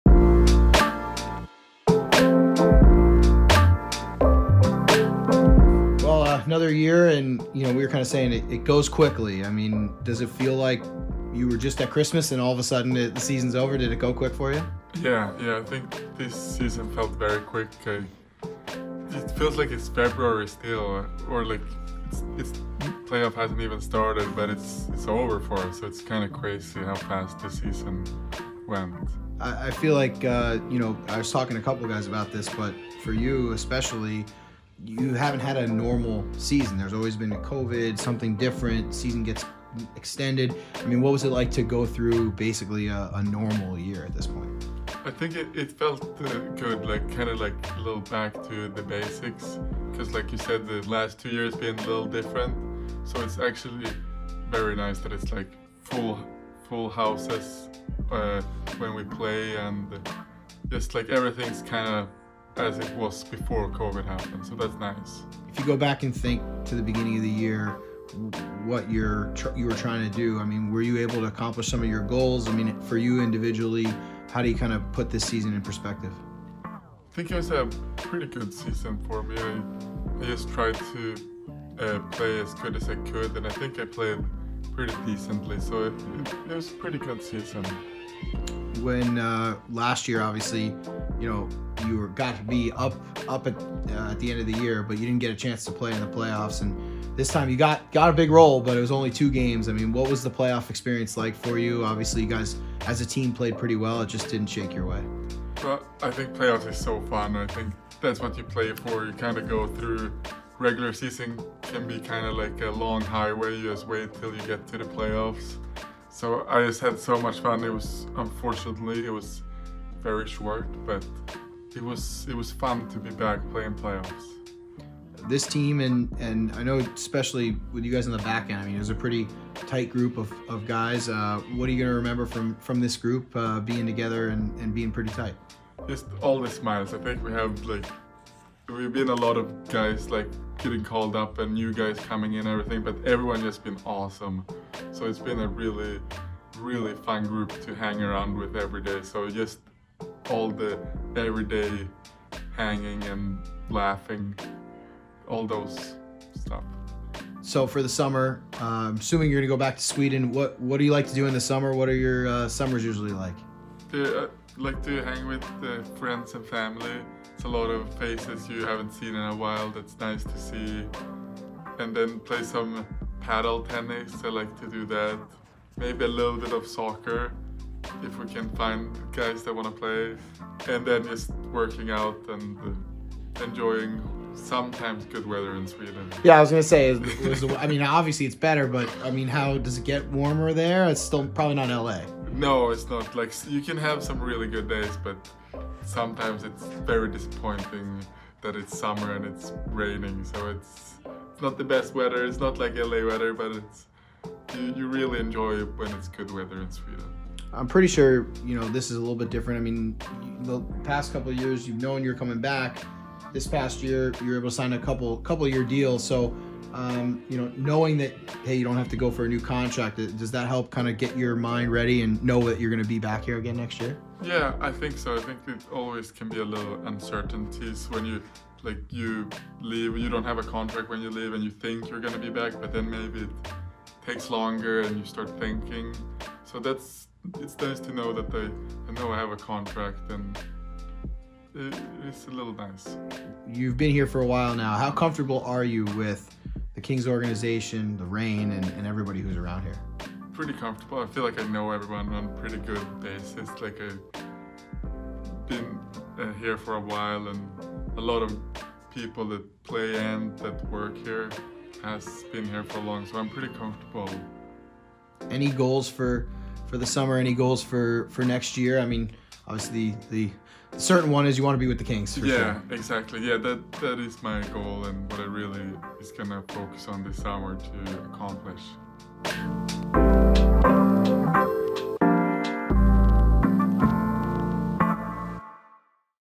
End of Season Interview - #43 Jacob Moverare
In our final end of season interview, Jacob Moverare discussed the return to a normal season and what he accomplished this past year.